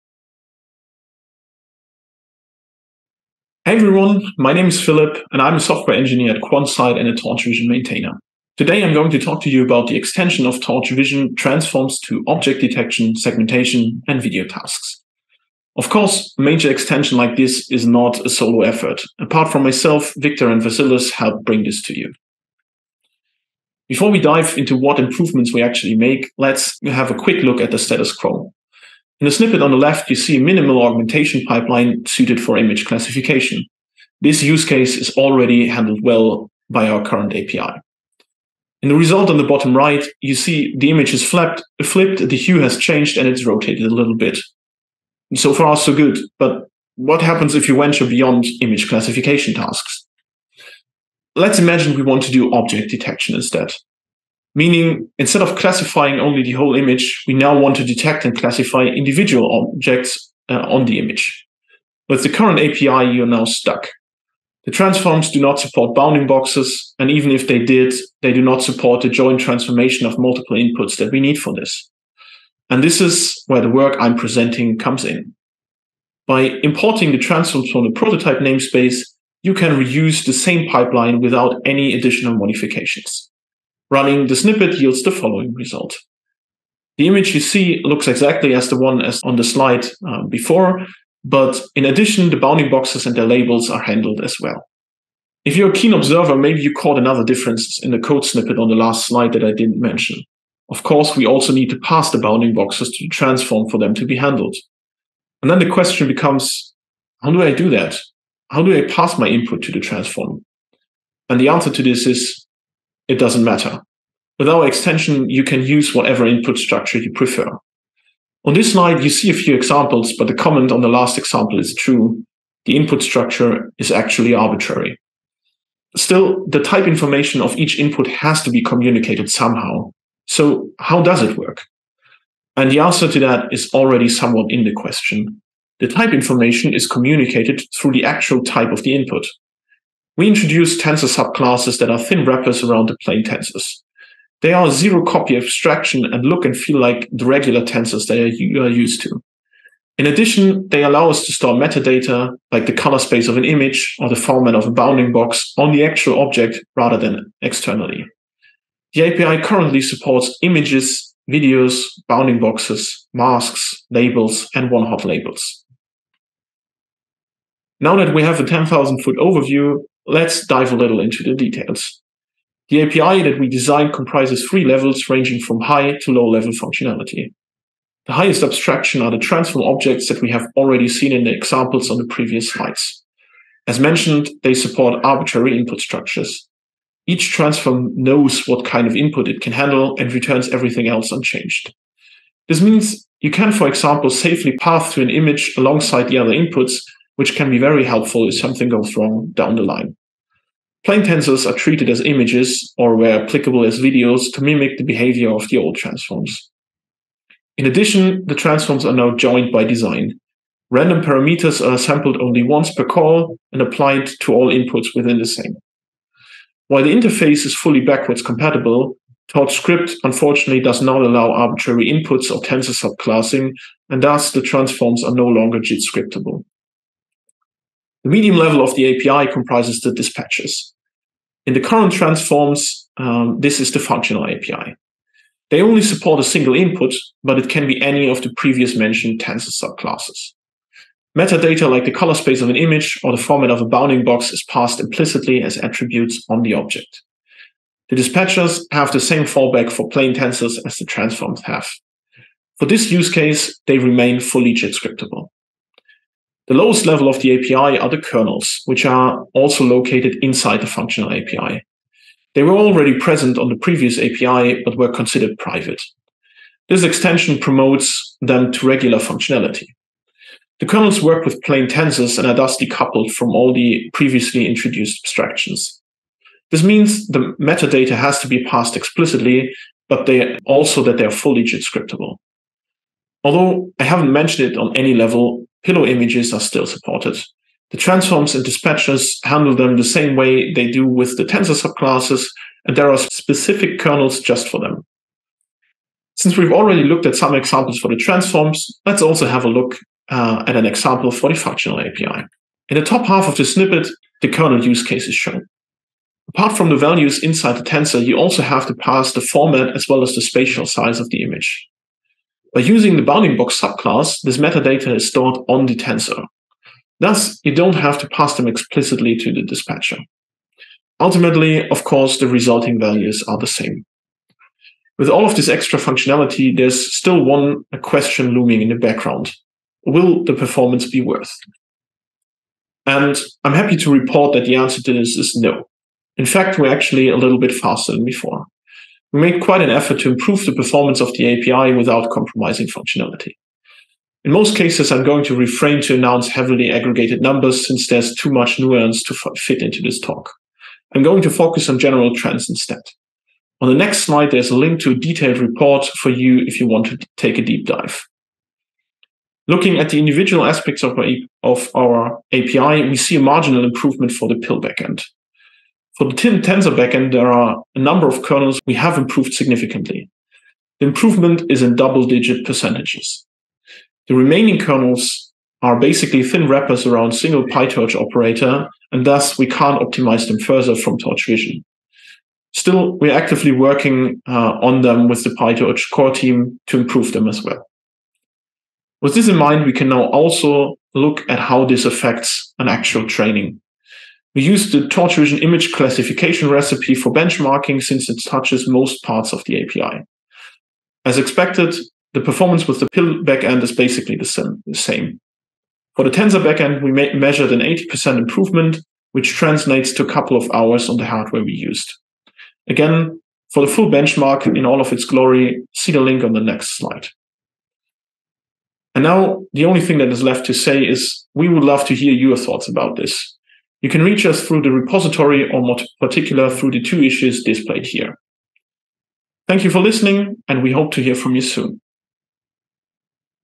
Torchvision Transforms
Philip Meier from Quansight presents "Torchvision Transforms" at PyTorch Conference 2022.

TorchVision is extending its Transforms API! This talk previews the current prototype that is no longer limited to Image classification, but can also natively handle Object Detection, Instance and Semantic Segmentation, and Video classification.